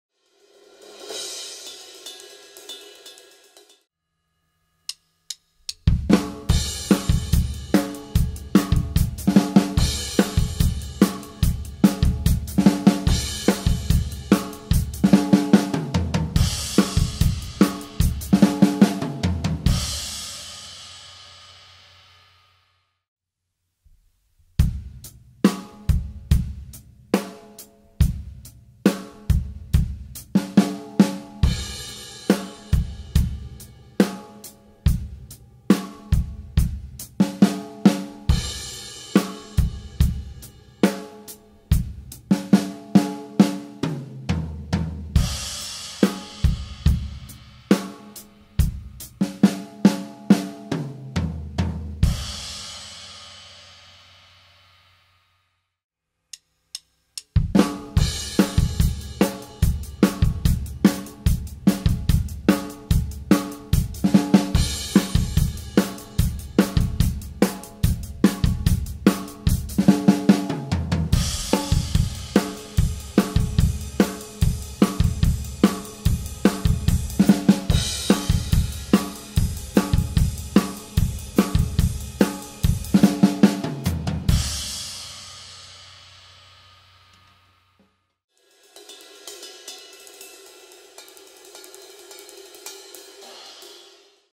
Herta #21 - The Easy Country Rock/Pop Hertas (Drum Fill Lesson)
In this drumfill lesson "Herta #21 - The Easy Country Rock/Pop Hertas" I play herta based drum fills in 16th notes and 8th notes. They work nice in many country, rock or pop situations.
In this drum lesson series you can find a lot of herta-based drumfills and drumlicks.
I like this kind of drum fills and licks. It's fun to play.
I wish you all the best and a lot of fun with your drumset!
Chris